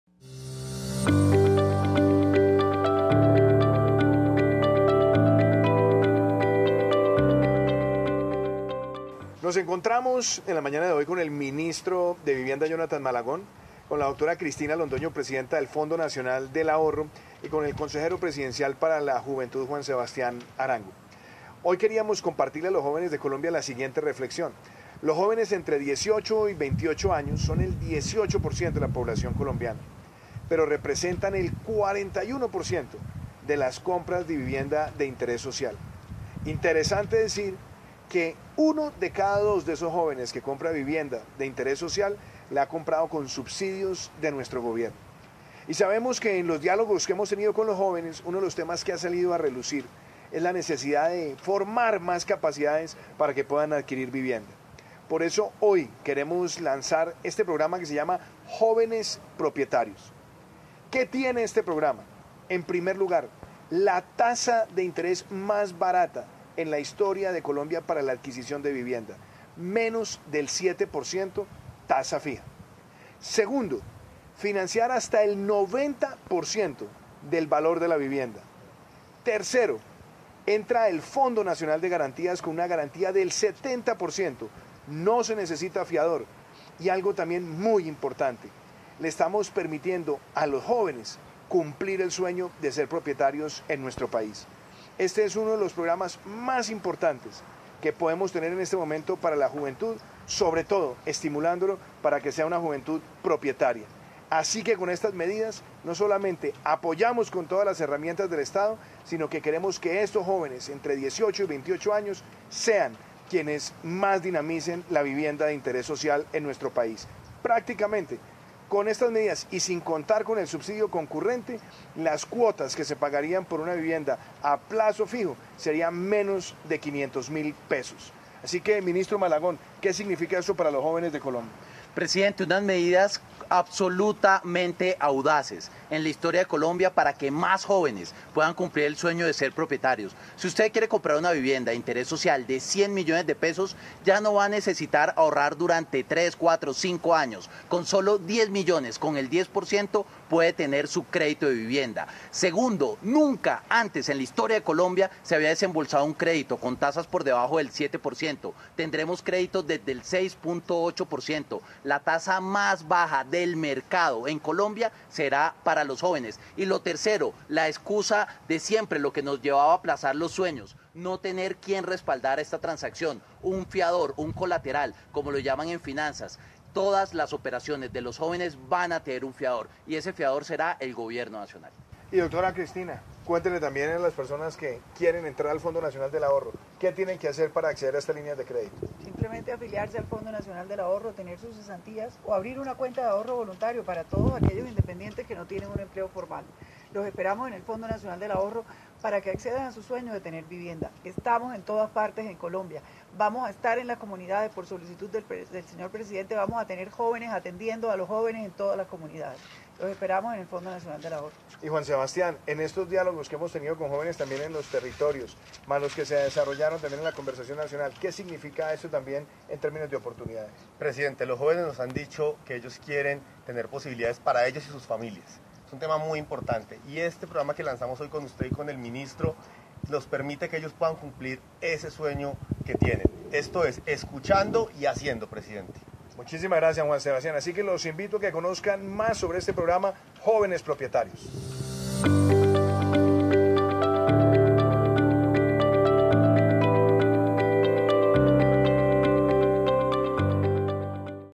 Presidente Duque y el Ministro de Vivienda, Jonathan Malagón sobre programa ‘Jóvenes Propietarios’
Declaración del Presidente Iván Duque y el Ministro de Vivienda, Jonathan Malagón sobre el lanzamiento programa ‘Jóvenes Propietarios’ - 18 de mayo  de 2021